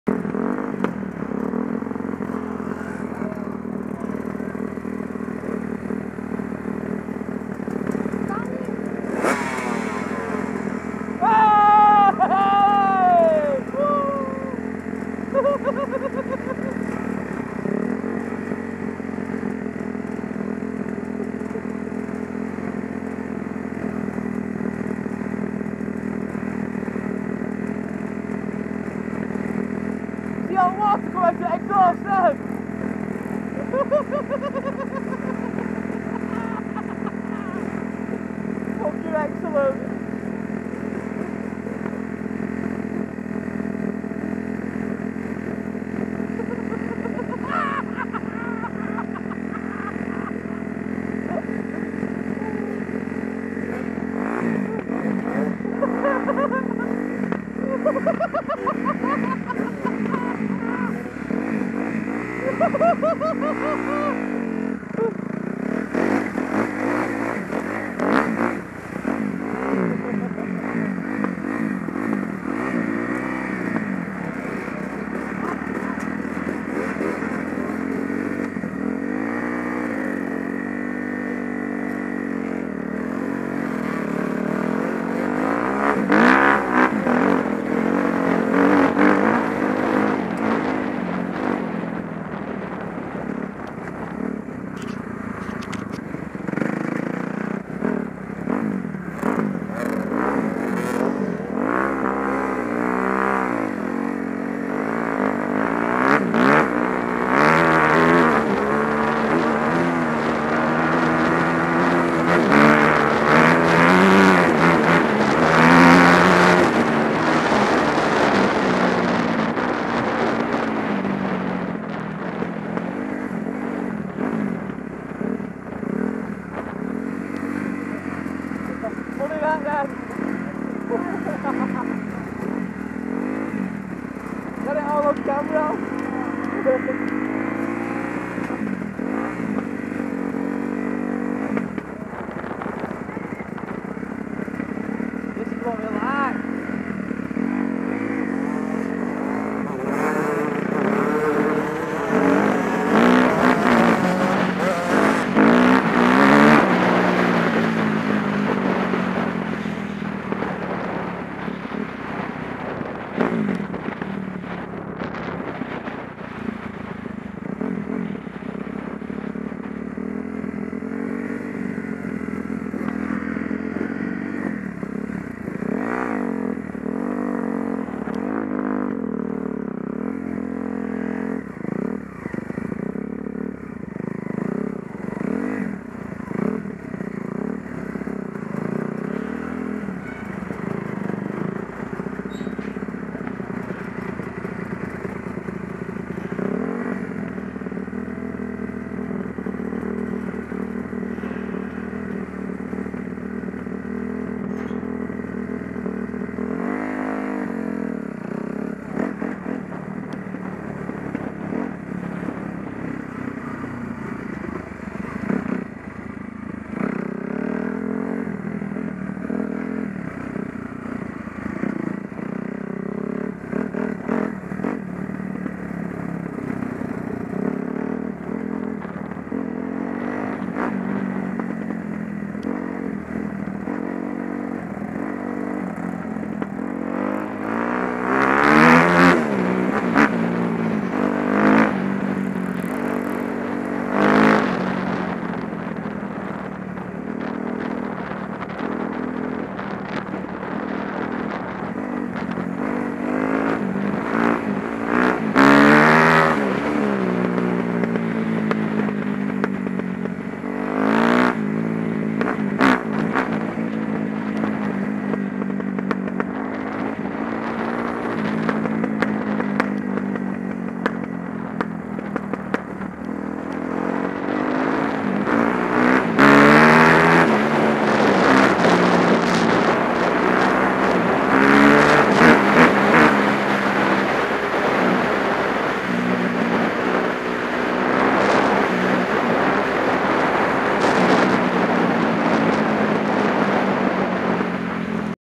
How not to cross a stream !! :D peak district enduro
How not to cross a stream !! :D peak district enduro. having a bit of fun on the bikes in the peaks